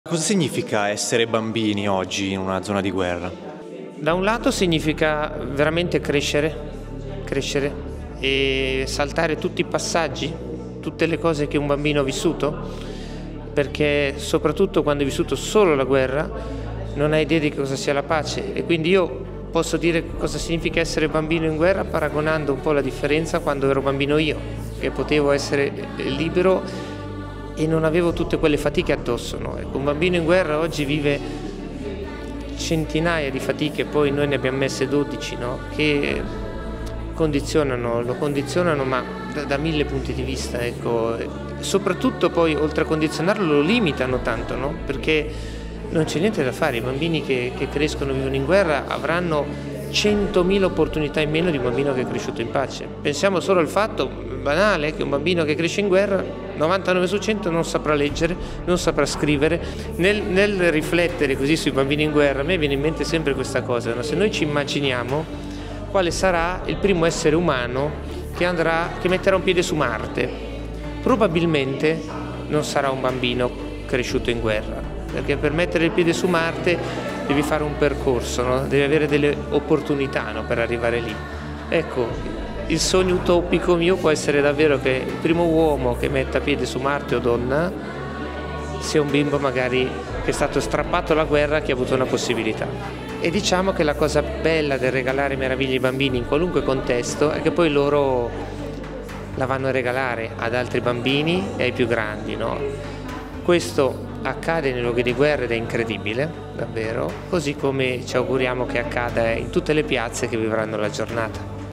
Varese - Il Pimpa, “Un bimbo sotto le bombe è senza futuro”
“Crescere sotto le bombe toglie ogni libertà ai bambini di diventare grandi” Il Claun il Pimpa ha viaggiato in lungo e in largo. Ha visto dal vivo le bombe, quelle di Gaza, del Donbass, dell’Iraq, della Siria dove ha portato un sorriso a tanti piccoli che avevano dimenticato persino cosa significa giocare. Il 13 ottobre torna la Giornata della Meraviglia, la sua giornata che anno dopo anni si sta espandendo in tutto il mondo. Quella stessa meraviglia sarà l’unica salvezza per tanti bimbi che tra paura e sofferenza provano ogni giorno a vivere una vita normale.